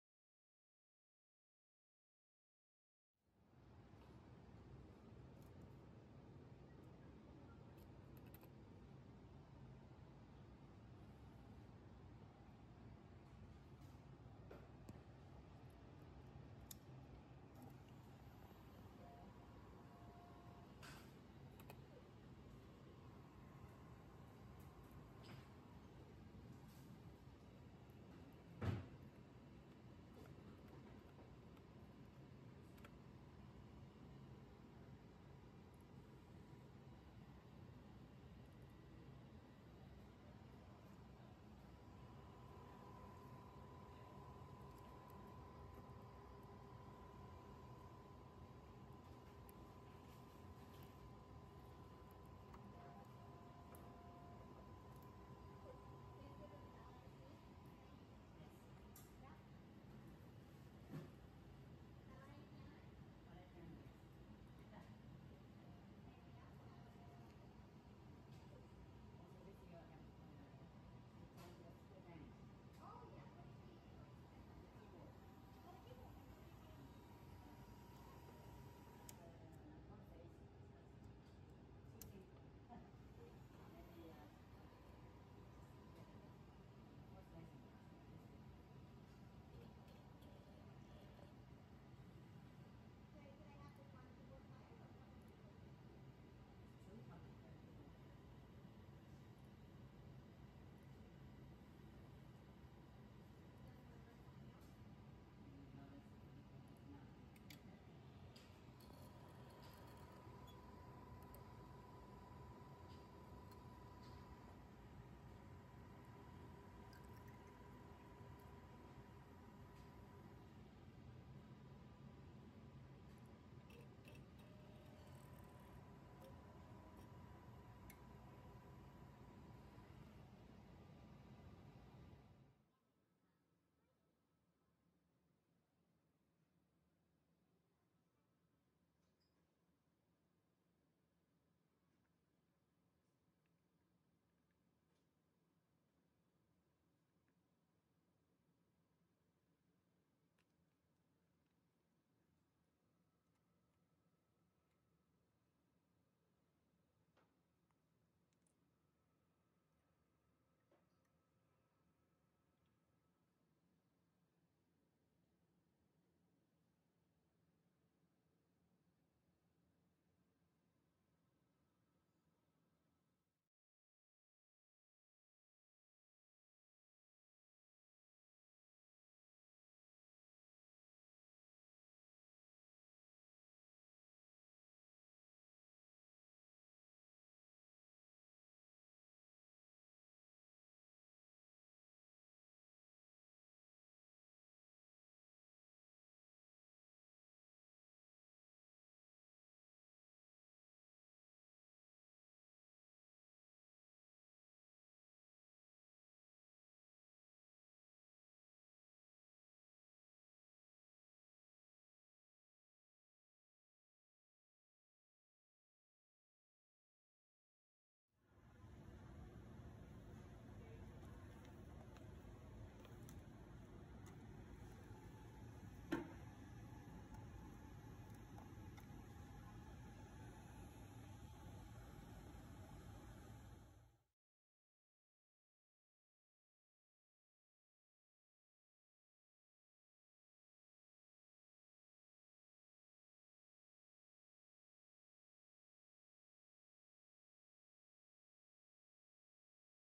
CytoFlex S Video Guide
Daily Startup, cleaning and QC video guide